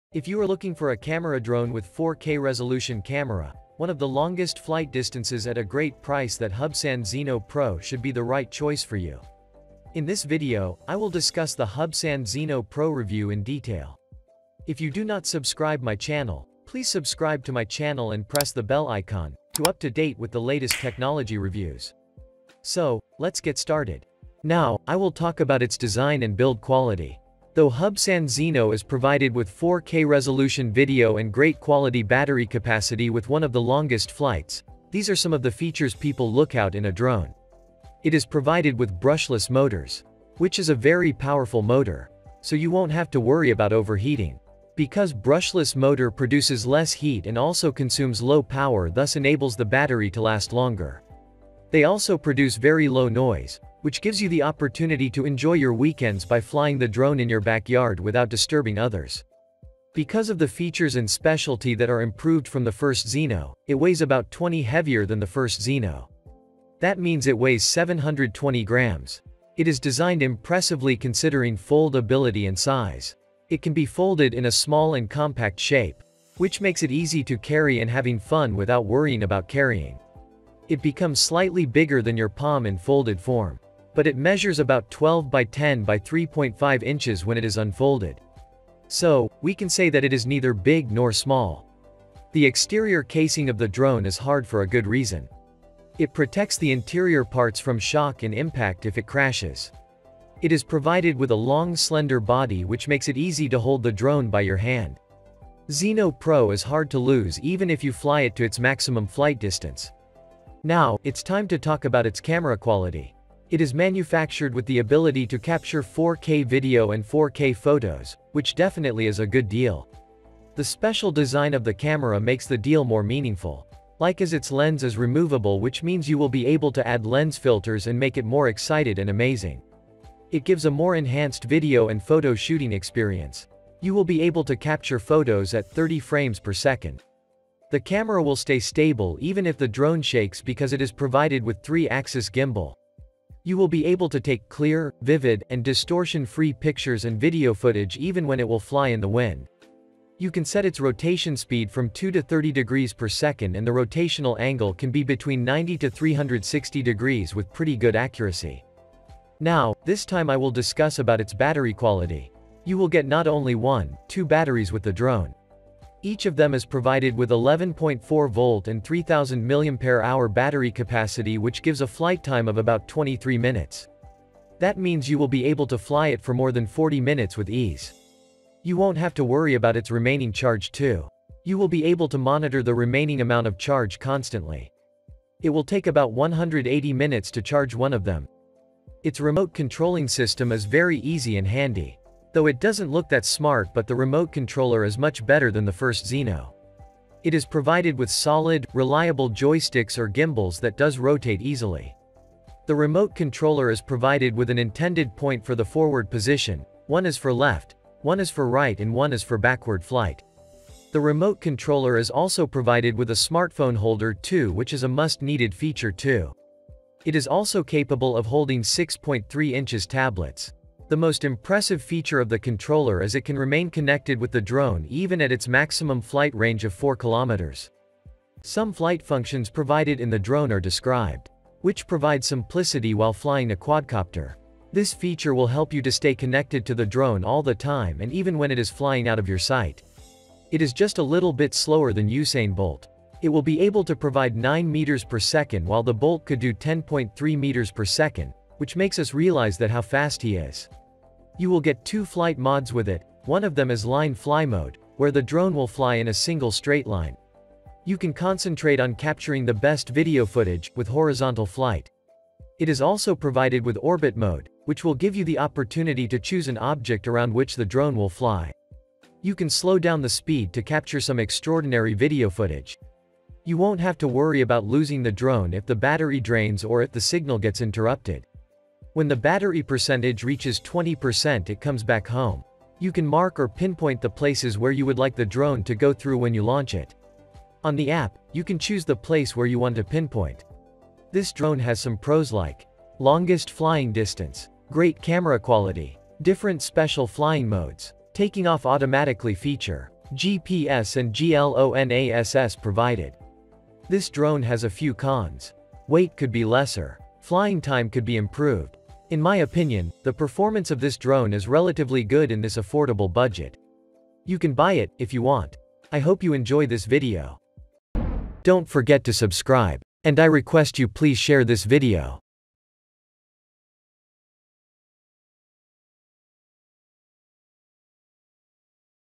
Best Professional 4K HD Drone on Aliexpress | Best Budget 4K Professional HD Drone 2024
All Products Link in the First Comment!

There is a lot of drone on the market. HUBSAN Zino Pro one of the best drone right now, in this video. I gave the full review of HUBSAN Zino Pro drone. I already discussed all features, camera quality, and also all pros and cons. So i hope you enjoy this video.